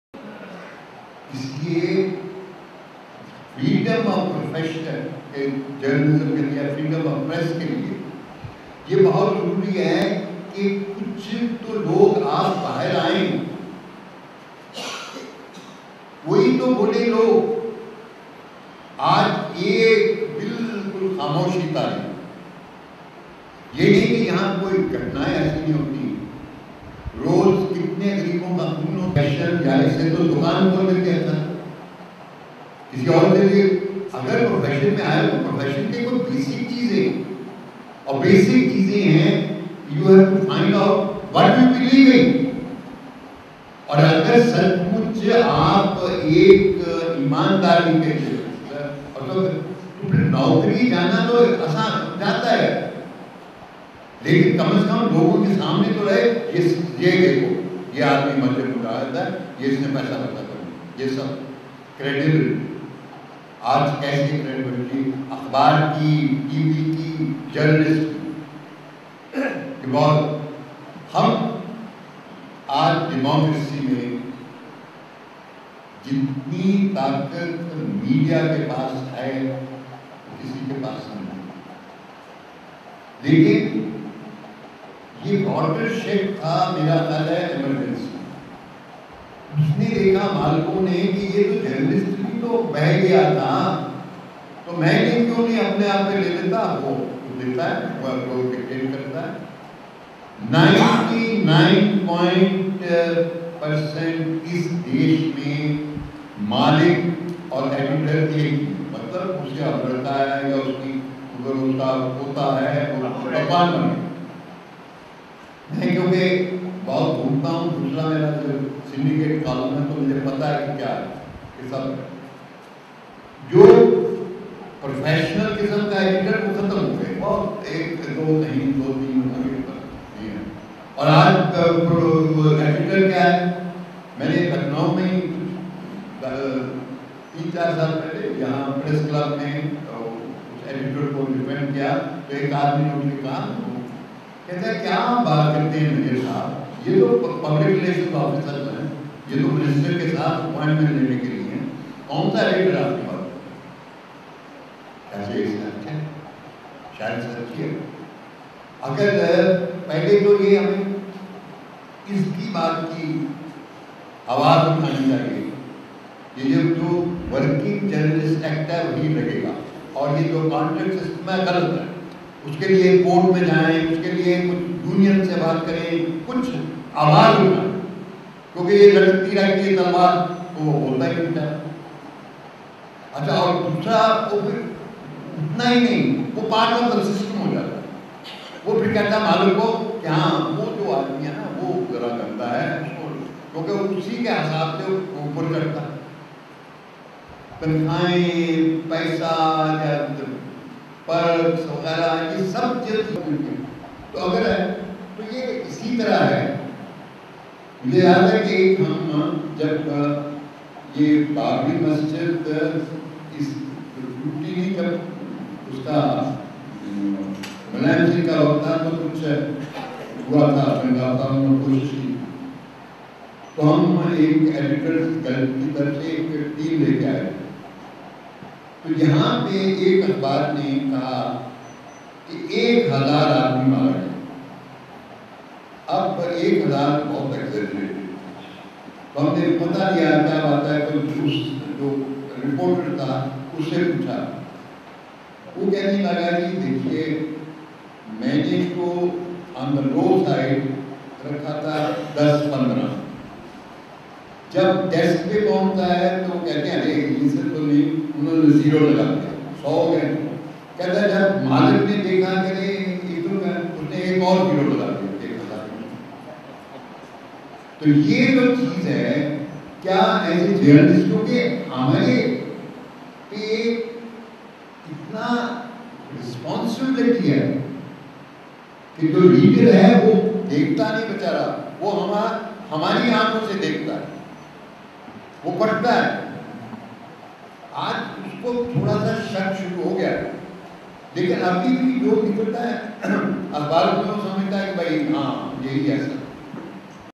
Kuldip Nayar's talk at Jahangirabad Media Institute on sting operation part 3
Jahangirabad Media Institute has hosted 3rd anniversary  celebrations of Media Nest on 13th November 2010. Legendary Indian journalist Mr.Kuldeep Nayyar inaugurated the function. There was heated discussions on "Sting Operations" and Kuldeep Nayyar strongly opposed the use of Sting operations in media. He said it is ethically, professionally and morally wrong to use Sting Operation as a tool of journalism. The editors of Times of India, India Today and correspondent of BBC were some of the media celebrates who have participated in the event. Mr.Khaja Hussain and Biju Mohan were also addressed the gathering on behalf of JET and JMI. Media has given wide coverage for the event.